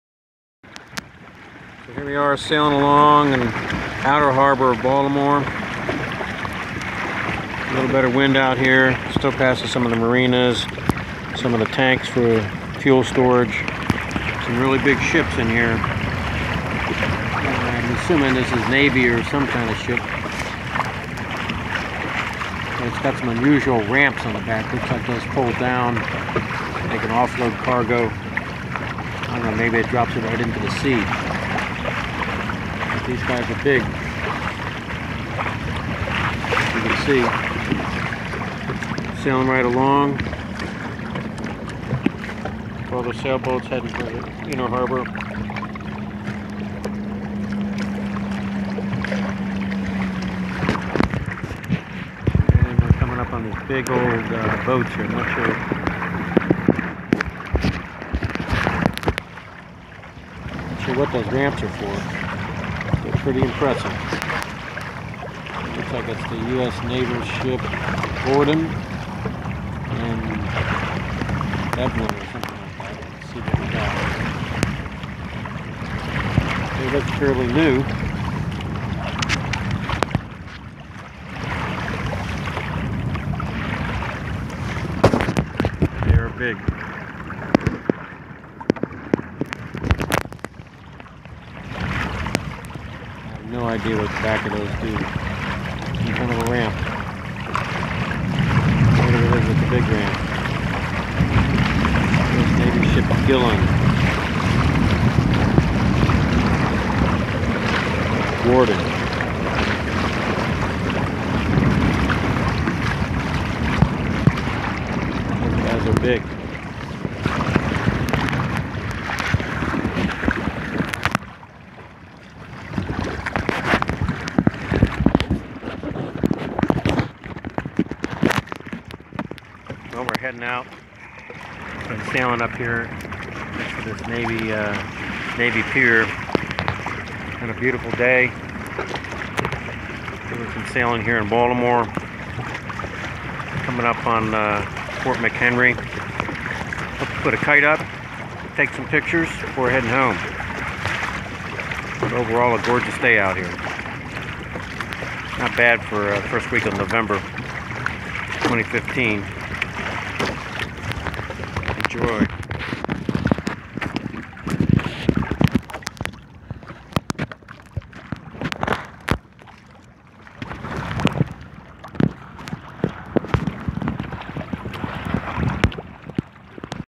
Hobie Adventure Island in Baltimore Inner Habor
Sail into the Baltimore Inner Harbor on a Hobie Adventure Island